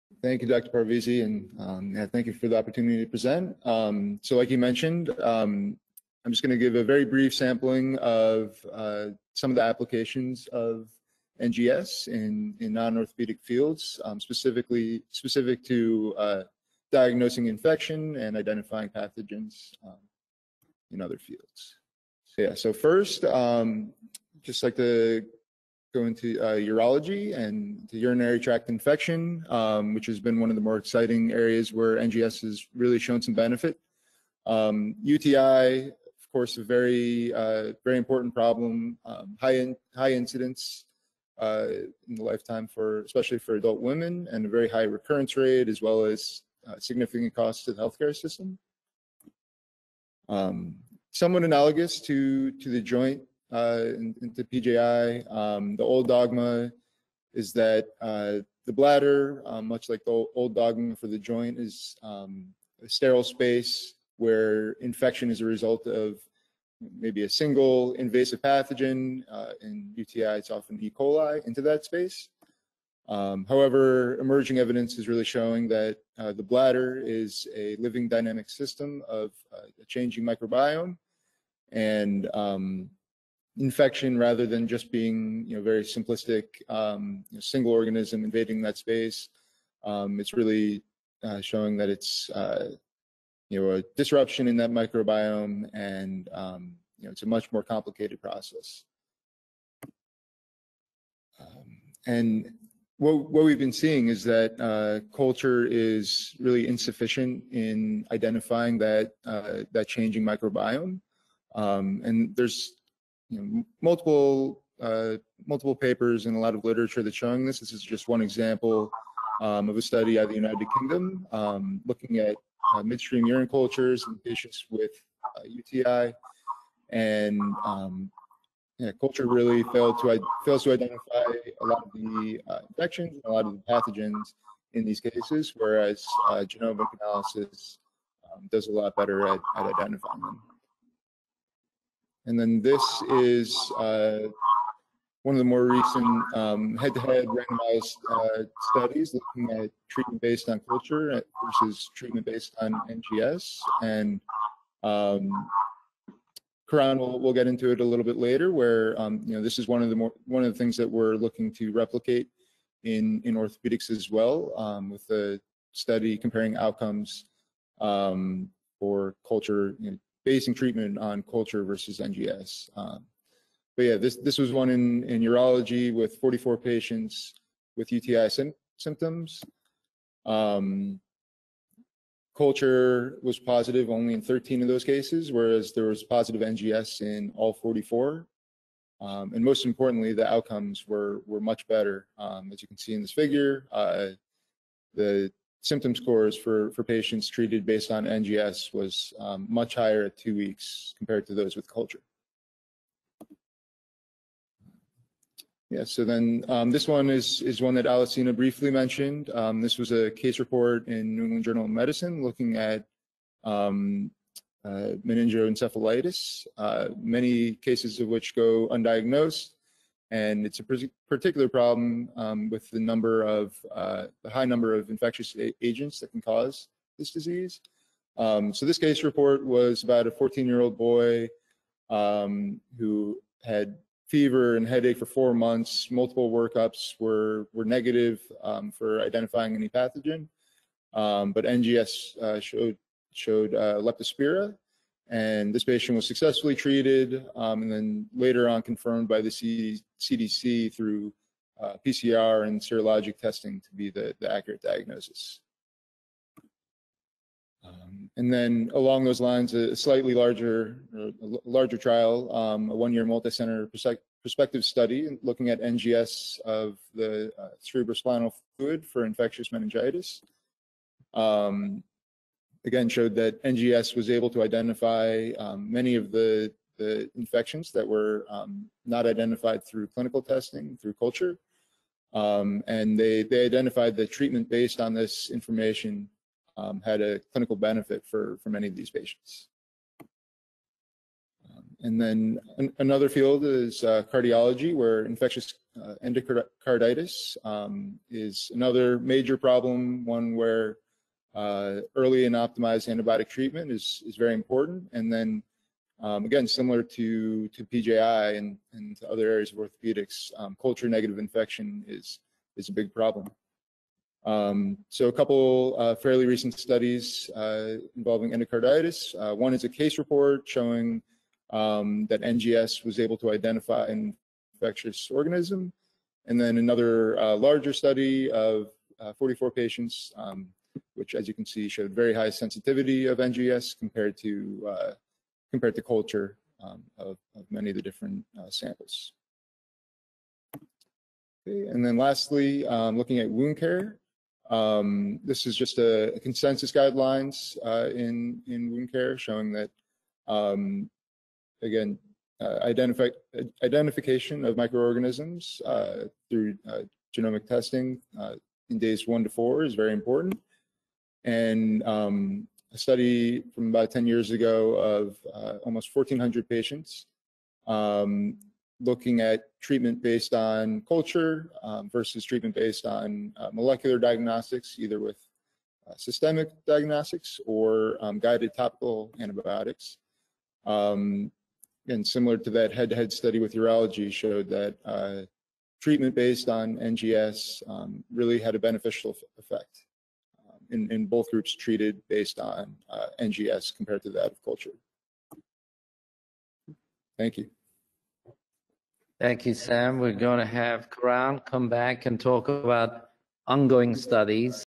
NGS in Non Orthopedic Fields - Samuel Clarkson, MD
This video is a recording of Dr. Javad Parvizi, who gathered experts from around the USA to discuss the use of DNA Next Generation Sequencing (NGS) to help diagnose and treat infections.  Dr. Parvizi held two events in April with these experts and had lively give and take, discussing Orthopedic Infections and DNA Next Generation Sequencing (NGS).

Dr. Parvizi is an Orthopedic surgeon associated with James Edwards, Professor of Orthopedic Surgery, Sidney Kimmel School of Medicine, and Rothman Institute at Thomas Jefferson University.

Listen as Dr. Parvizi brings in experts and discusses the uses and benefits he and the other panelists have found using DNA Next Generation Sequencing (NGS) to treat their patients with severe infections successfully.